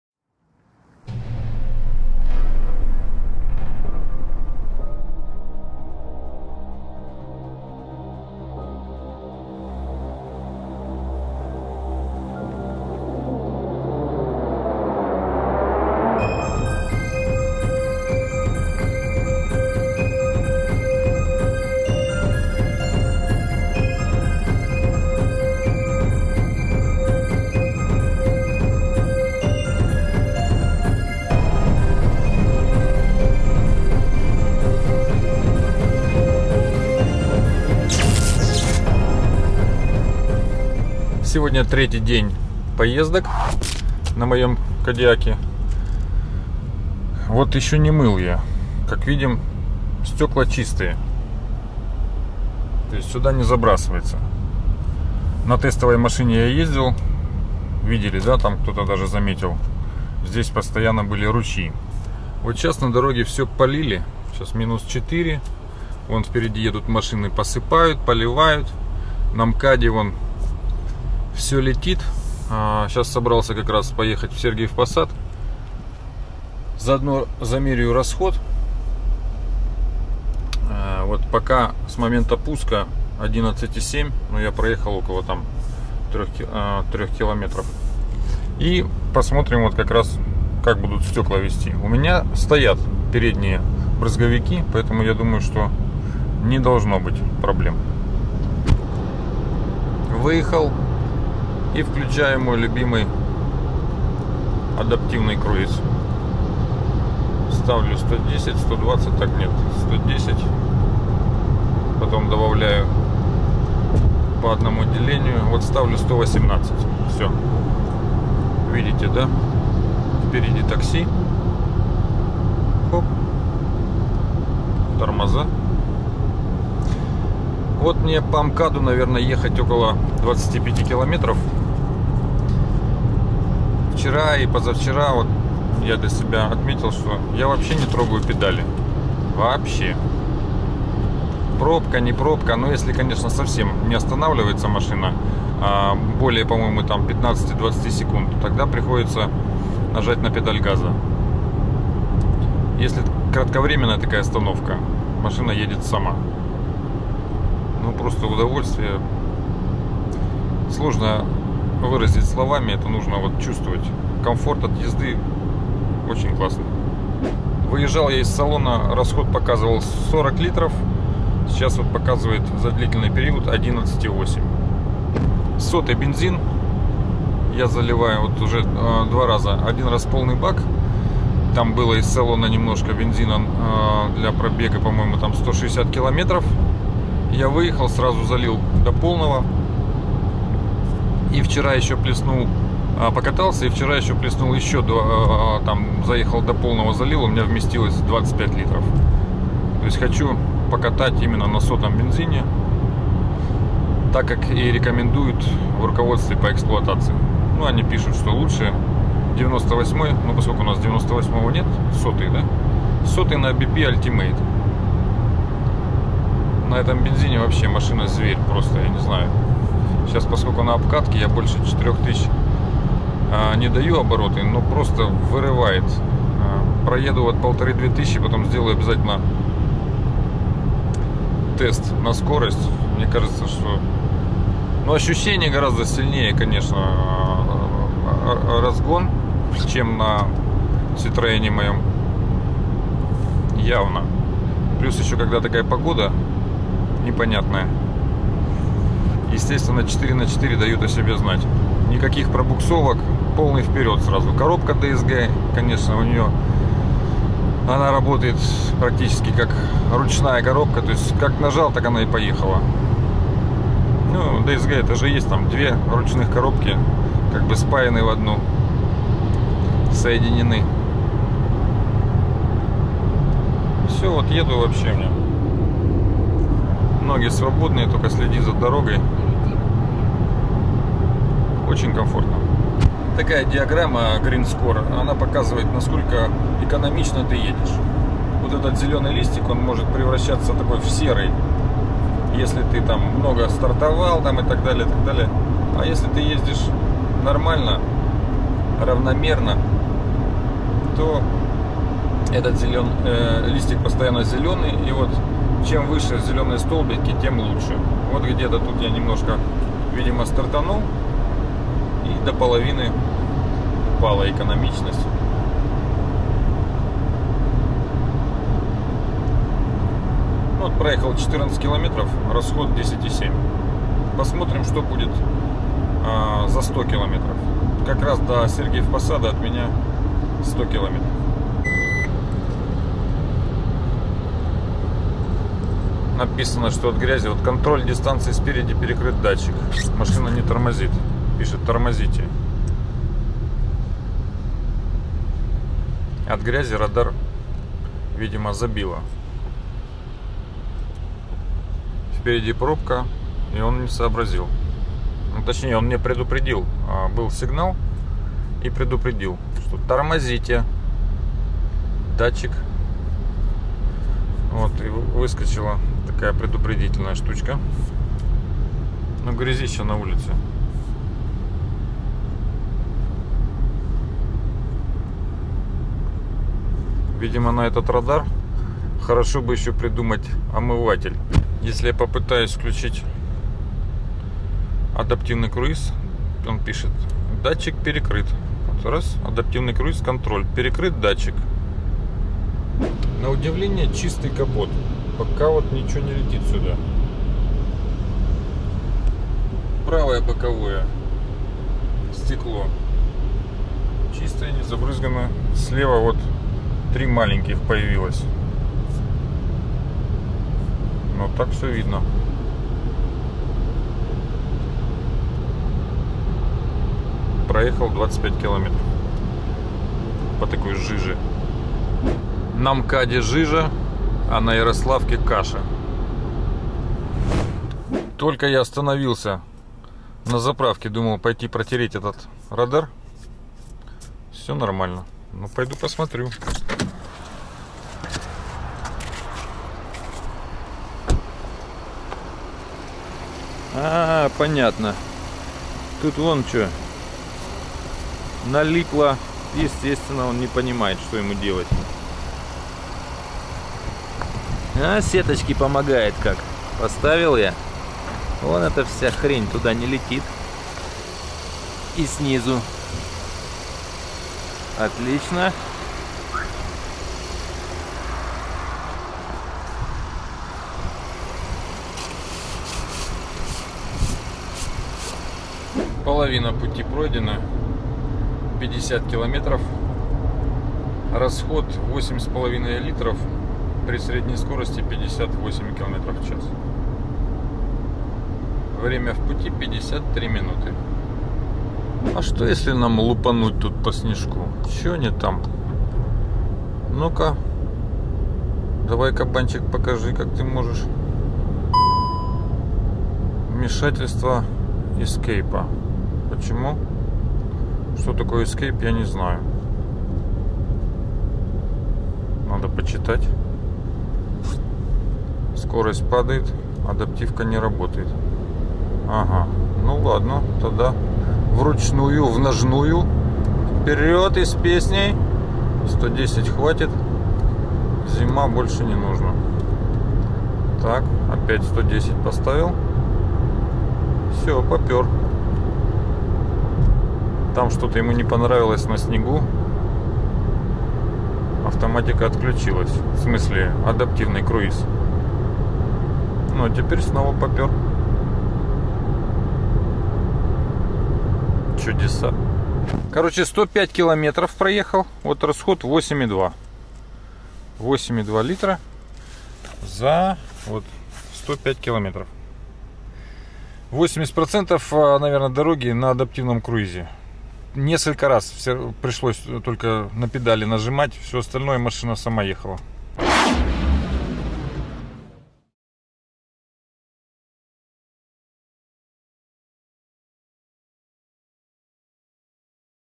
Мой Кодиак. Выпуск 01 || Поехали... расход, круиз, бензин
Этим выпуском я начинаю рассказ об эксплуатации моей новой машины Шкода Кодиак 2.0 TSI 180 л.с. DSG7 4х4 2019 года выпуска.
Видео о машине Шкода Кодиак (Skoda Kodiaq) 2019 года выпуска в комплектации Style с мотором TSI  2.0 180 л.с. российской сборки.
Куплена в Москве в начале 2019 года за 2,4 млн рублей
Дополнительные пакеты:
Пакет ассистентов Traffic Jam assist
Зимний пакет II
Функциональный пакет Style IV